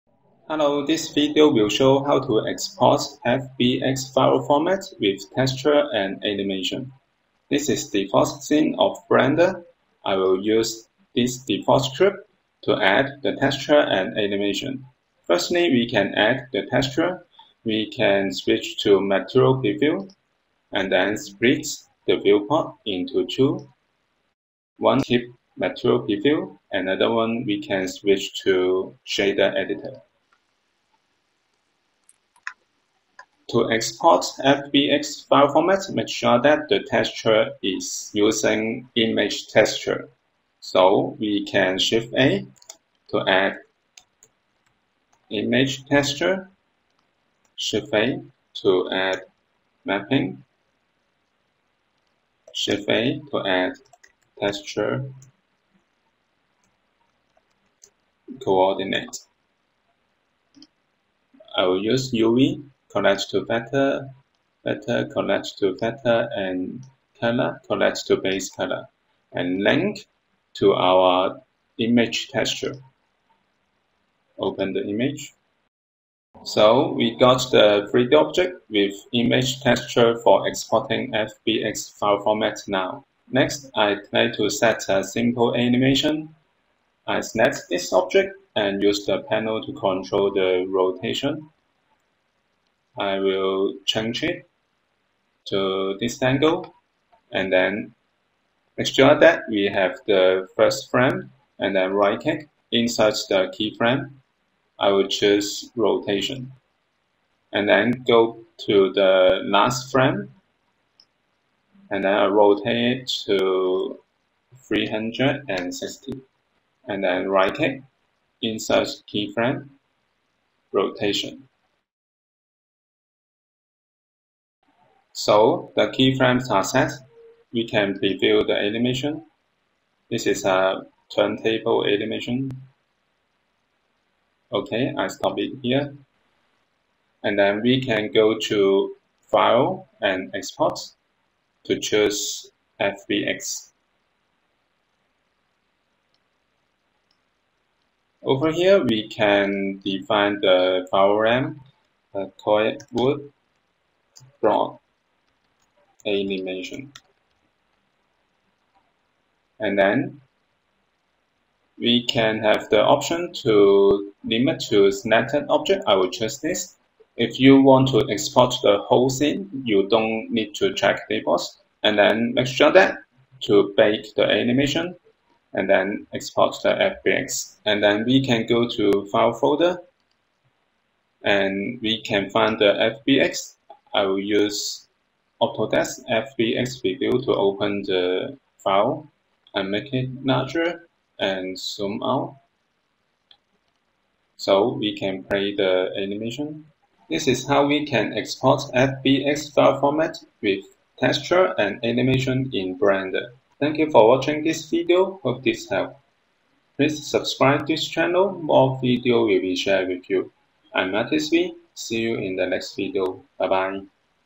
How to Export FBX with Texture and Animation in Blender
If you find that exporting FBX file texture is missing, here is the tutorial video to show how to export FBX with texture and animation. In this video, we use default cube for example, we add wood texture into 3D object with using image texture node, and then add animation keyframes in timeline. Then, we can export FBX embedded texture and animation. Finally in this video, we can see how to check FBX file with Autodesk FBX Review. Hope this helps.

Timestamp

0:00 Intro – Export FBX with texture and animation
0:19 Add Image Texture for FBX
1:28 Add Animation Keyframes
2:16 Export FBX with Texture and Bake Animation
3:00 Check file with Autodesk FBX Review